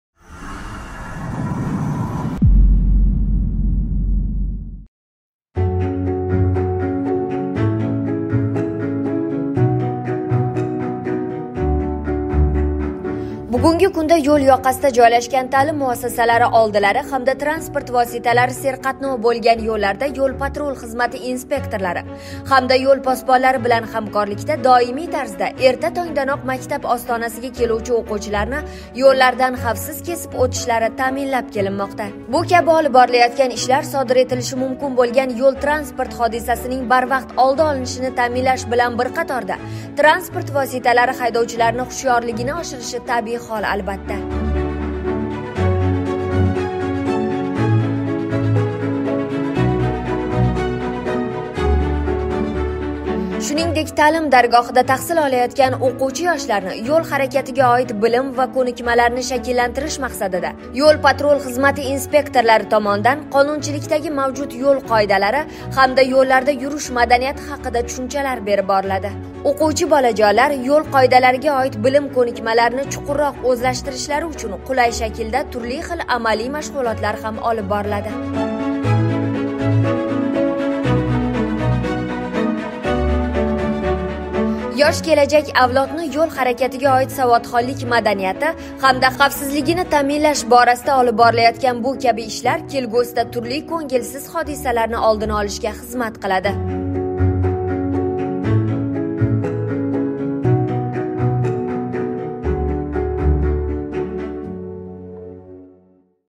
Harakat xavfsizligini ta’minlash borasida YPX xodimlariga yo‘l posbonlari ham ko‘maklashmoqdalar
Shuningdek, YHXB xodimlari ta’lim muassasalariga tashrif buyurib, bolalarga yo‘l harakati qoidalari bo‘yicha bilim berishda davom etmoqdalar.